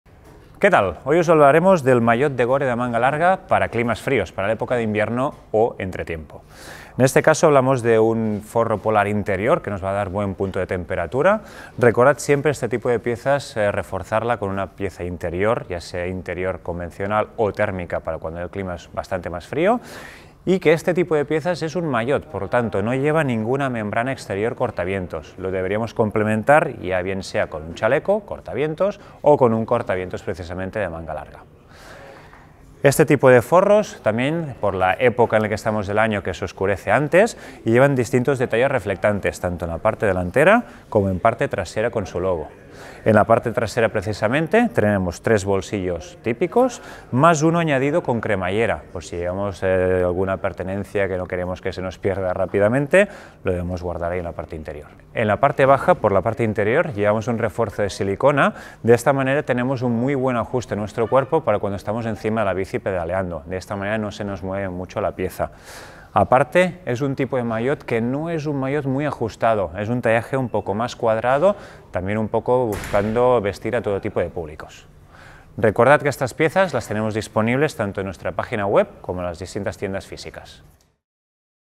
Gore C3 Thermo Jersey
El Maillot Manga Larga Gore C3 Thermo Jersey está confeccionado con un forro polar interior para los días de frío.

Este cálido maillot se ajusta a la posición de pedaleo. Puede llevarse como capa exterior o bien como capa intermedia bajo la chaqueta para una calidez superior.

Los logotipos reflectantes aseguran la visibilidad nocturna. Prenda imprescindible para pedalear en climas fríos.